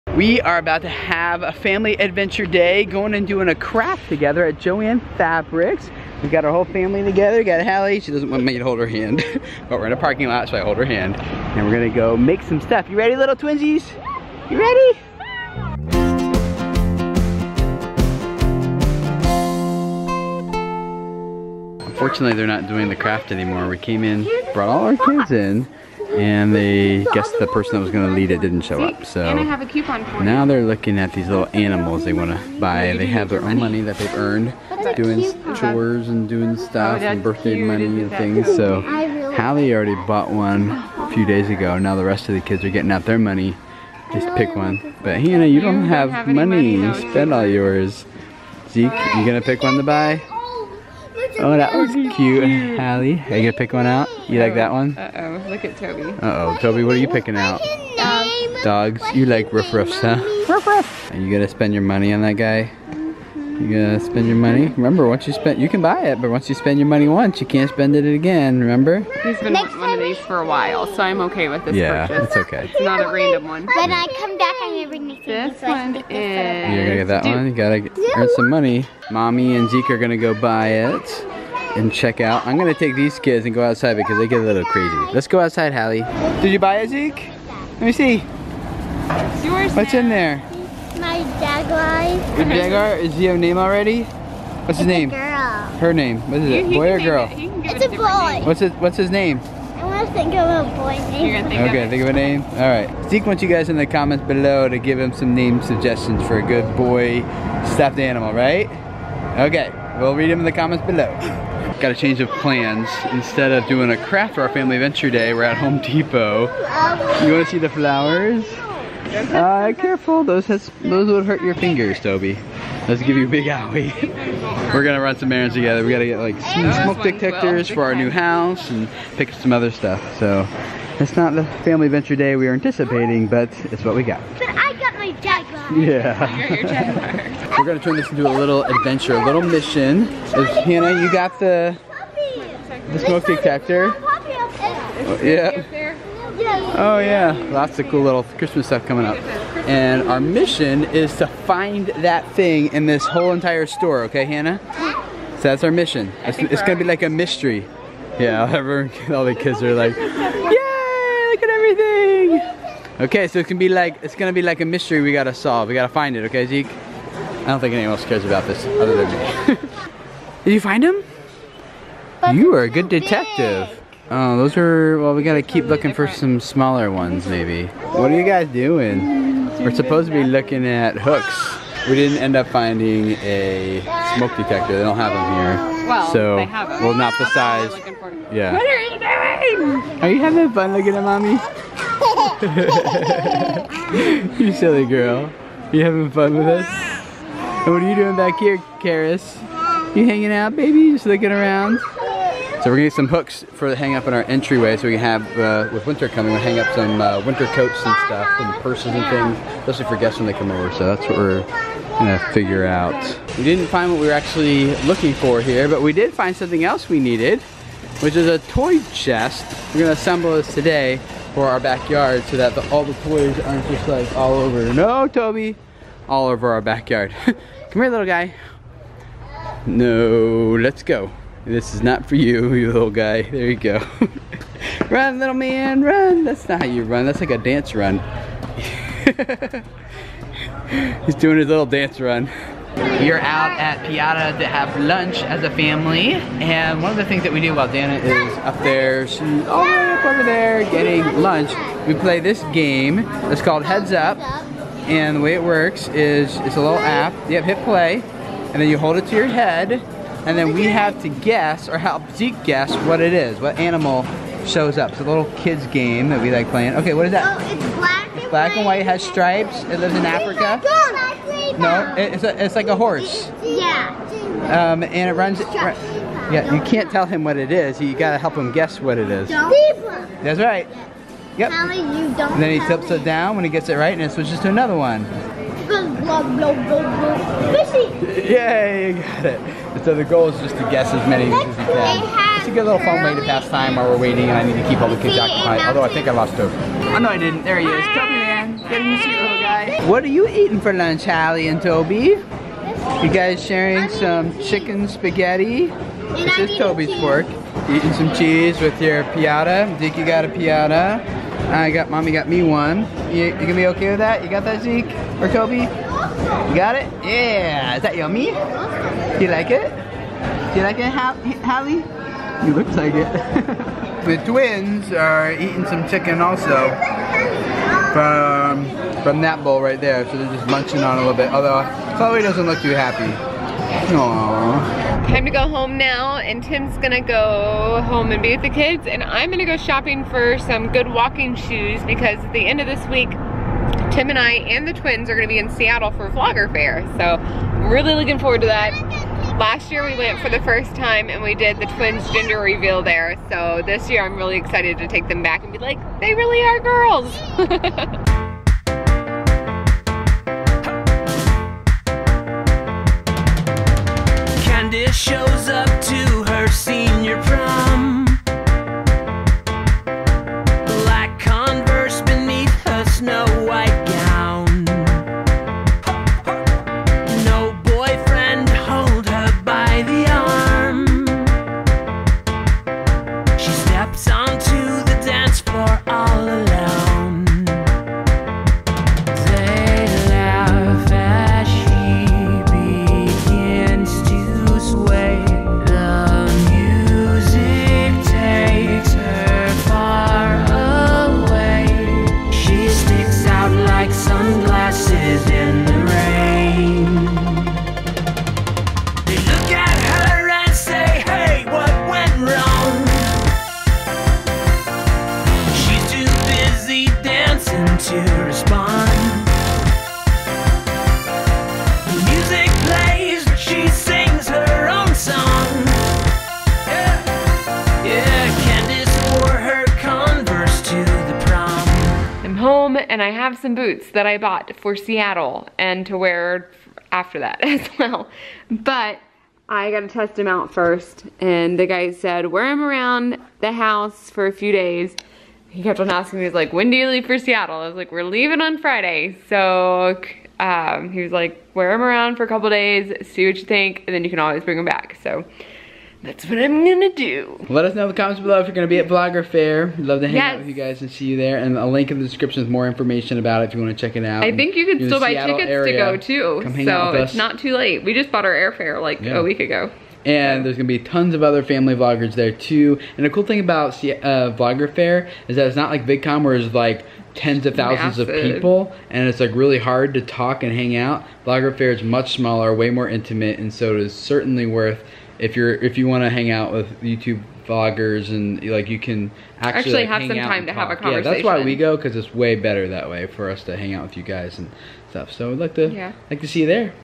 Shopping while Wearing TWINS! 🛍
Our family of 8 went out to have some fun together, but ended up shopping instead, including the two baby twins! Dana wore them both on her back while I juggled the others, trying to run some errands and buy some things we need at home. Then Dana got some alone time to go shopping for nice-looking comfortable shoes for VloggerFair.